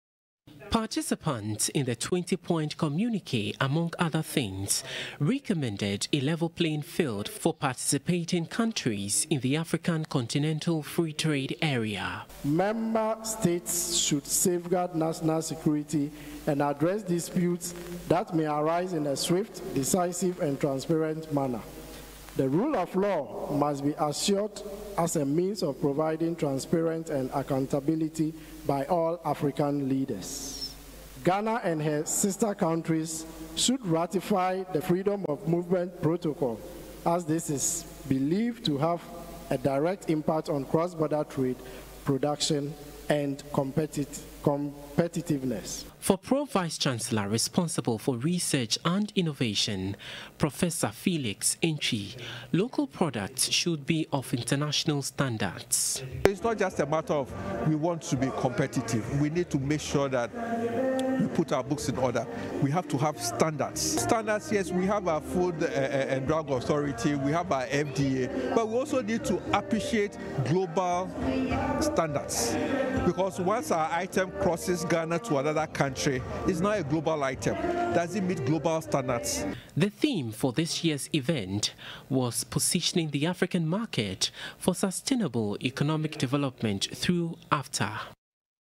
UG new year school: Development standards for AFCFTA: UG Pro Vice Chancelor.
Development standards for AFCFTA: UG Pro Vice Chancelor.